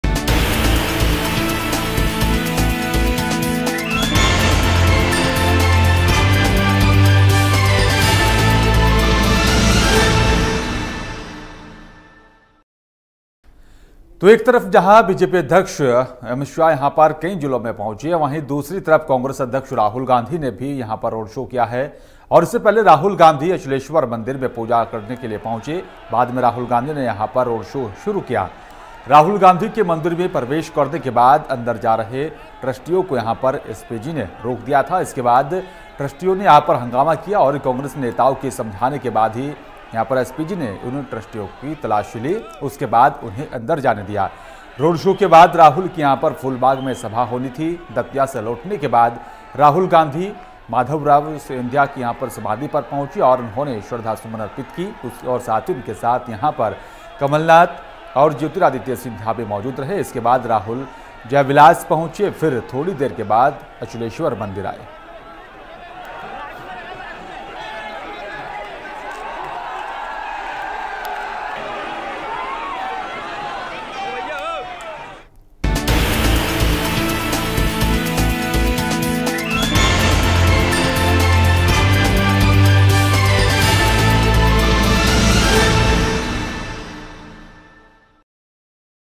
Madhya Pradesh Election: Rahul Gandhi ने किया ऐतिहासिक रोड शो
एक तरफ जहां बीजेपी अध्यक्ष एमपी के कई जिलों में पहुंचे वहीं दूसरी तरफ कांग्रेस अध्यक्ष राहुल गांधी ने भी रोड शो किया। इससे पहले राहुल गांधी अचलेश्वर मंदिर में पूजा की।  बाद में राहुल का रोड शो शुरू हो गया है। राहुल गांधी के मंदिर में प्रवेश करने के बाद अंदर जा रहे ट्रस्टियों को एसपीजी ने रोक दिया। इसके बाद ट्रस्टियों ने हंगामा कर दिया। कांग्रेस नेताओं की समझाने के बाद एसपीजी ने ट्रस्टियों की तलाशी ली और अंदर जाने दिया। रोड शो के बाद राहुल की फूलबाग में सभा होनी थी है। दतिया से लौटने के बाद राहुल गांधी माधवराव सिंधिया की समाधि पर पहुंचे और उन्हें श्रद्धासुमन अर्पित किए। उनके साथ कमलनाथ और ज्योतिरादित्य सिंधिया भी मौजूद थे। इसके बाद राहुल जयविलास पहुंचे। फिर थोड़ी देर बाद अचलेश्वर मंदिर आए।